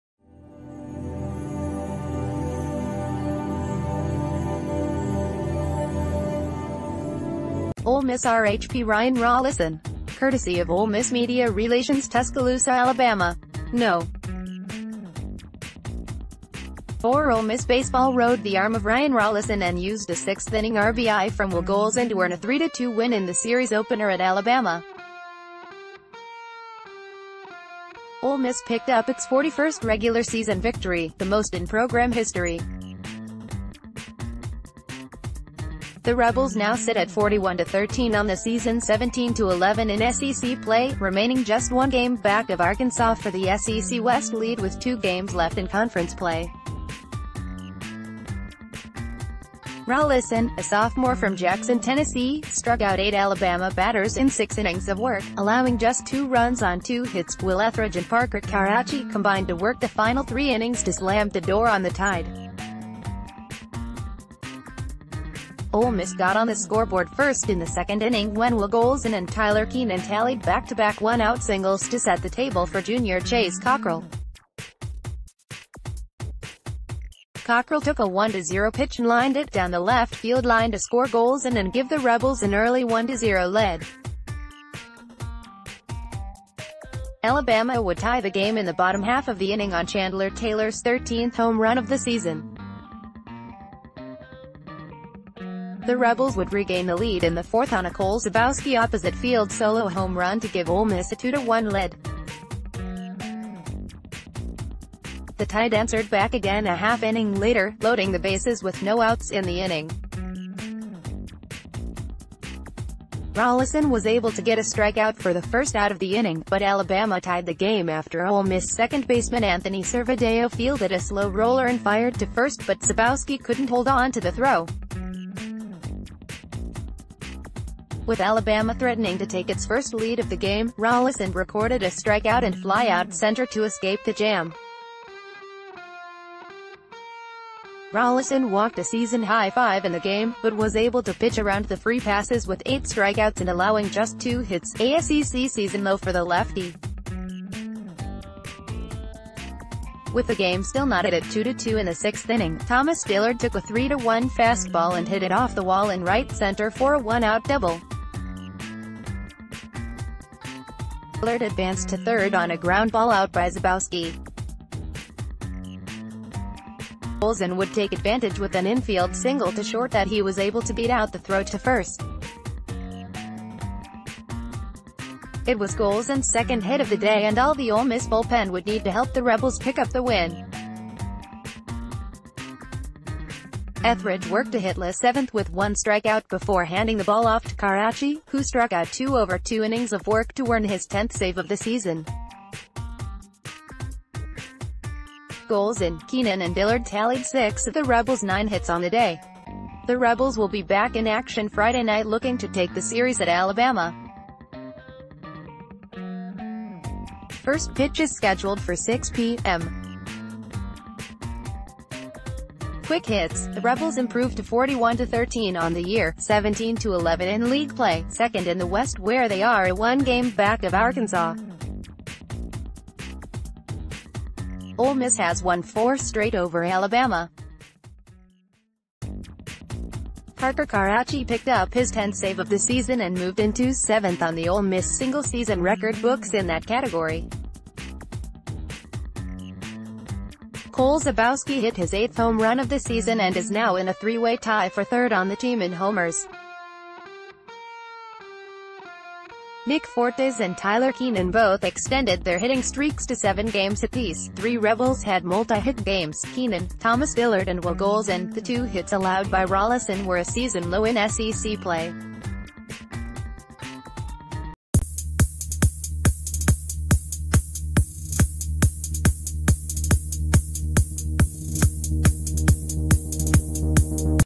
Breaking News | No. 4 Ole Miss baseball rode the arm of Ryan Rolison and used a sixth inning RBI fr
Breaking News | No. 4 Ole Miss baseball rode the arm of Ryan Rolison and used a sixth inning RBI from Will Golsan to earn a 3-2 win in the series opener at Alabama.

Ole Miss RHP Ryan Rolison. Courtesy of Ole Miss Media Relations TUSCALOOSA, Ala. – No. 4 Ole Miss baseball rode the arm of Ryan Rolison and used a sixth inning RBI from Will Golsan to earn a 3-2 win in the series opener at Alabama. Ole Miss picked up its 41st regular season victory, the most in program history. The Rebels now sit at 41-13 on the season 17-11 in SEC play, remaining just one game back of Arkansas for the SEC West lead with two games left in conference play. Rolison, a sophomore fr...